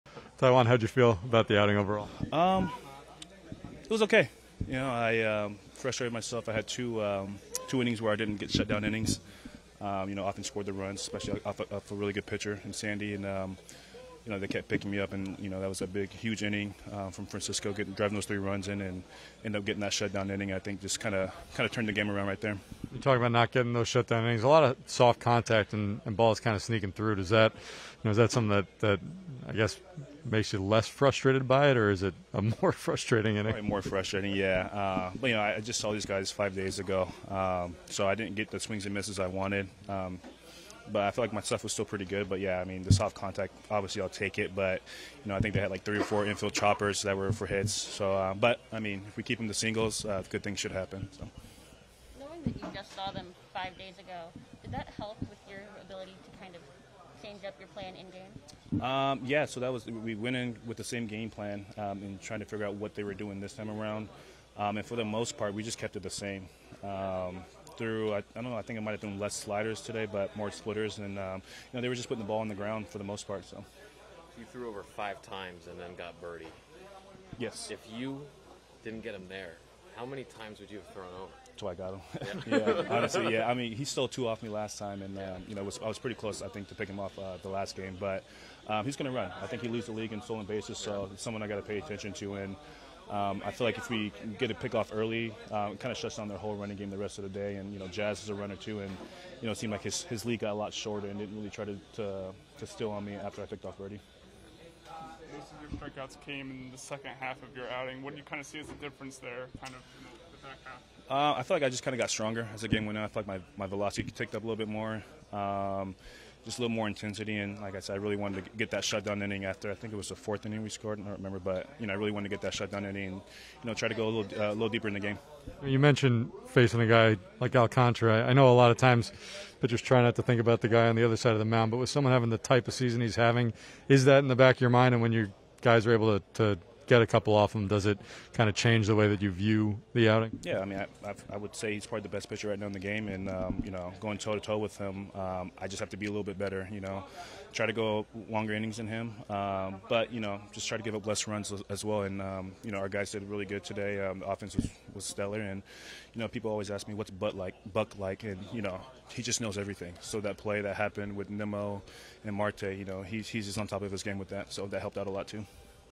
Taijuan Walker talks controlling Marlins running game, getting stronger as game progressed
In this Mets post game news conference,  Taijuan Walker felt picking off Miami's leadoff hitter Jon Berti in the first inning helped keep the running game under control. While Walker was a little frustrated with some soft contact hits, He opted to go with a similar game plan to the one he had when he faced them last weekend, throwing a few more splitters than sliders this time around. "I felt like I got stronger as the game went on."